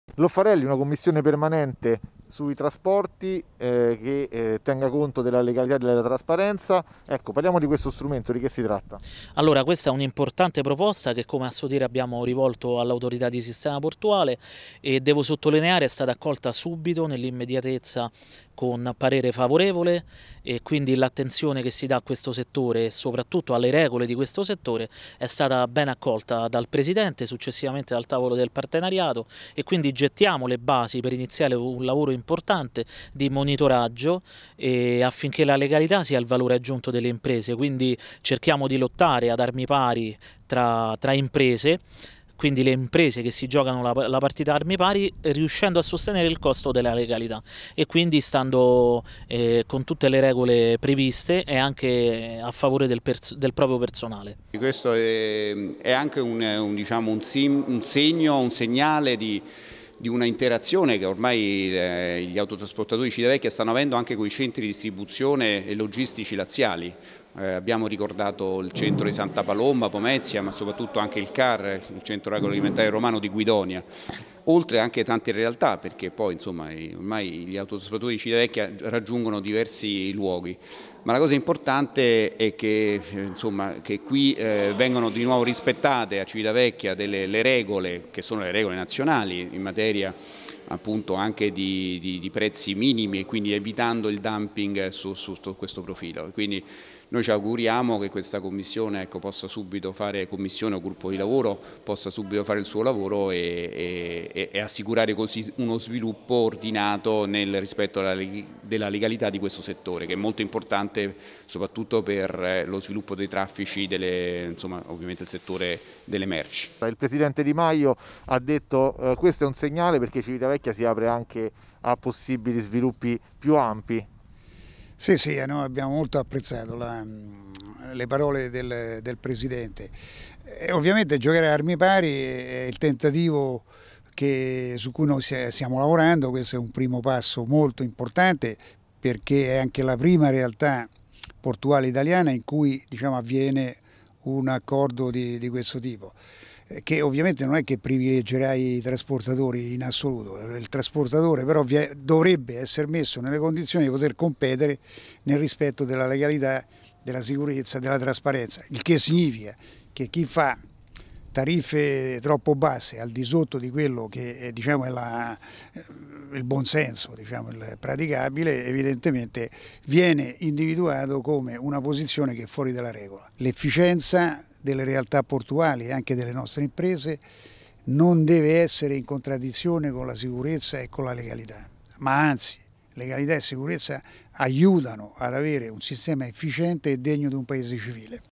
Autotrasporti, Adsp e Cac uniti per legalità e trasparenza
Una commissione permanente per la trasparenza e la legalità nell'autotrasporto delle merci. E' il risultato ottenuto ieri mattina in un incontro tra Autorità Portuale e Consorzio Autotrasportatori Civitavecchia.